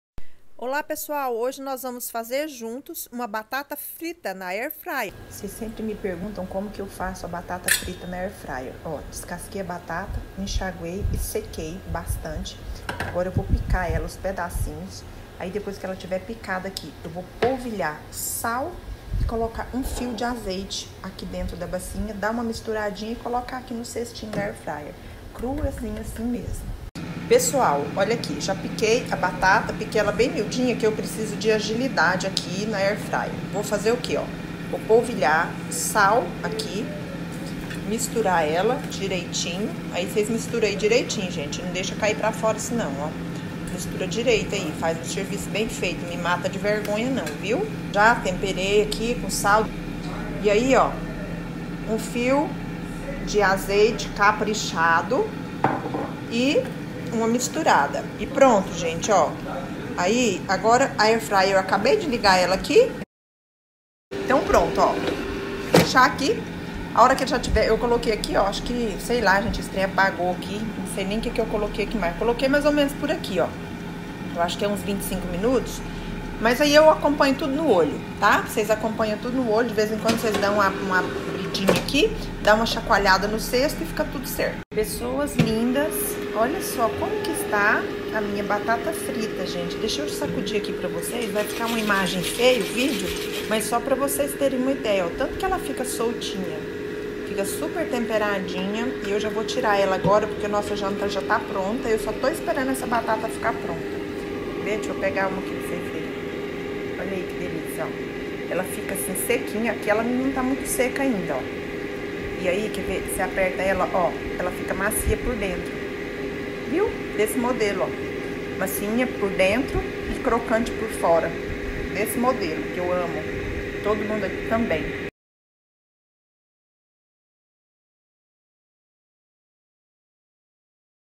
BATATA FRITA CROCANTE SEM ÓLEO
Caso não tenha a  ayr fryer faça esse mesmo processo no forno do fogão a 200graus